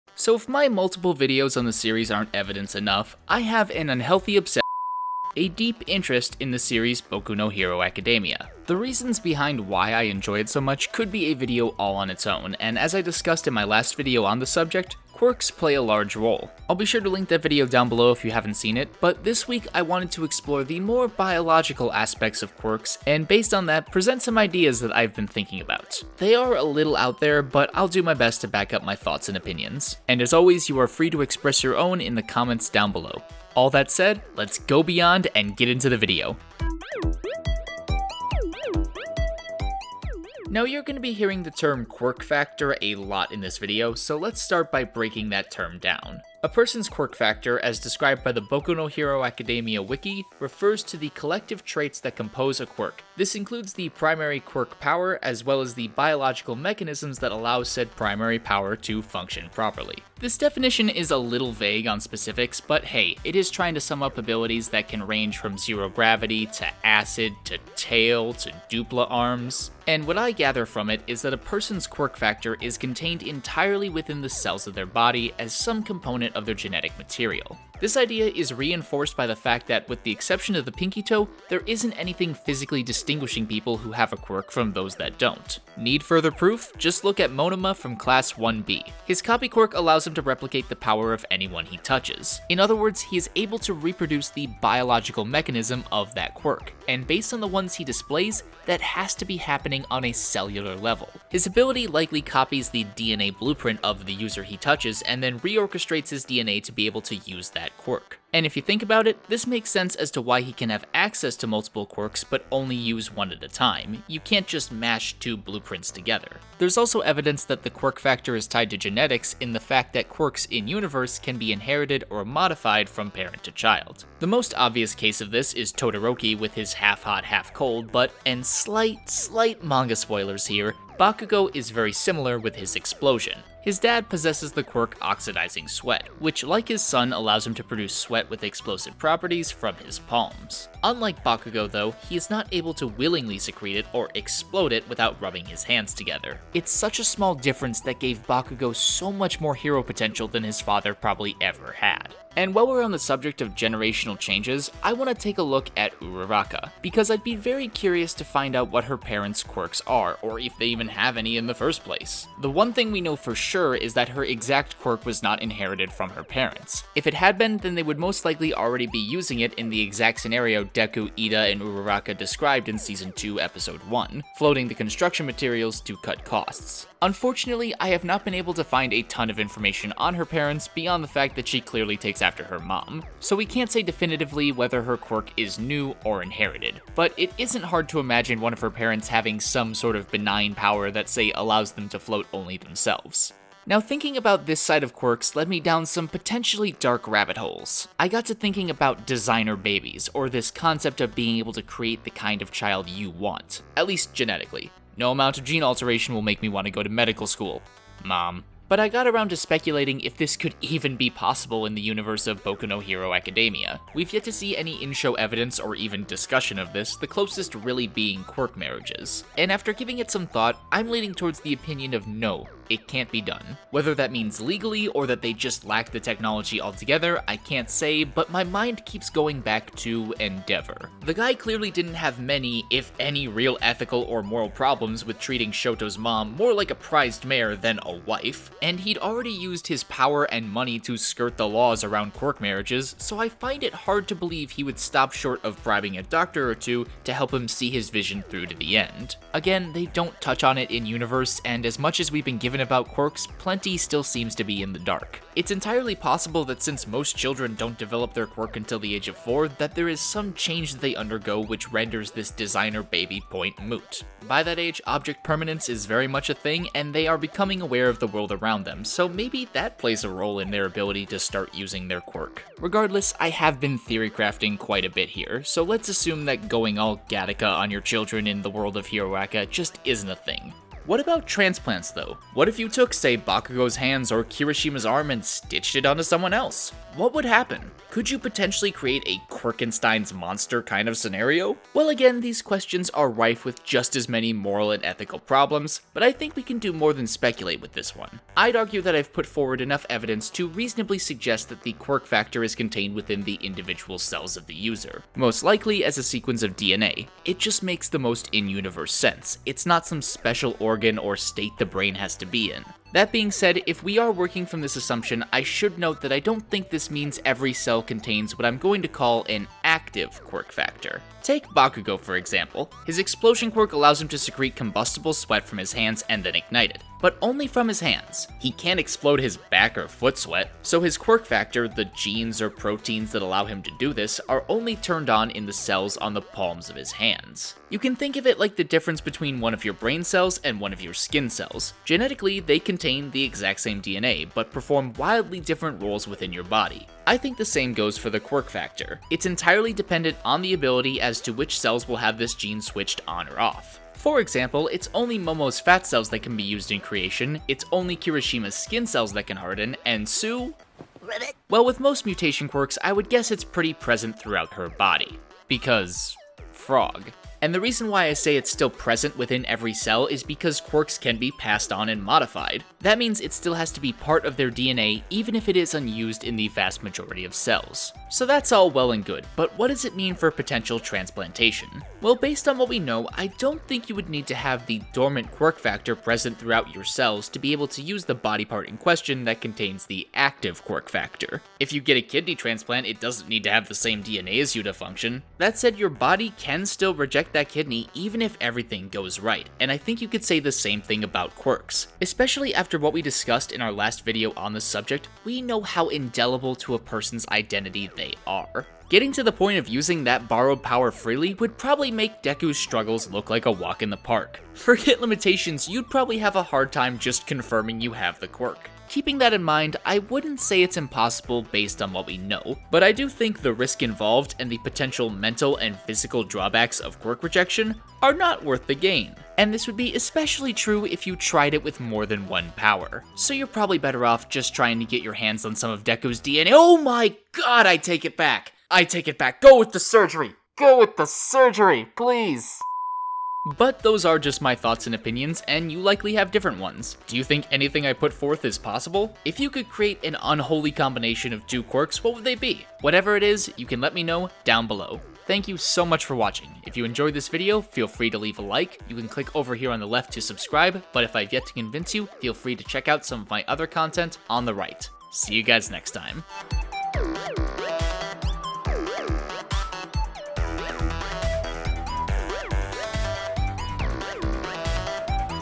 Powers in HeroAca Pt. 2 - Quirky Boogaloo
Hello, everyone! We're back yet again with Boku No Hero Academia. because I have a problem... anyway this week I wanted to look back into Quirks and the biological side of them, because as someone with an interest in genetics and biology, there is quite a bit of things to dig into in-universe.